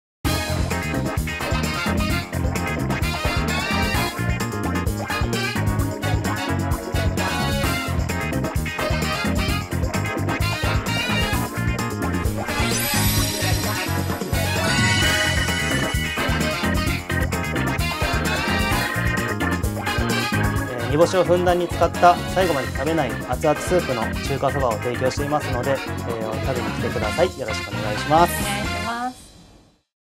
上北沢『中華そば はな田』ラーメンWalker
店主の花田さんはなんと、歌舞伎町のホストだったという異色の経歴。そんな彼がホストを辞めてまで追い求めたのは、今はなき新大久保の伝説店『めとき』の味。三角形の海苔や挽肉で和えたメンマなど、その端々に『めとき』らしさを感じさせる一杯は、まさに花田店主による“めときオマージュ”。上北沢の地でご夫婦で紡ぐ味、ぜひご堪能を。

詳しくはこちらをチェック！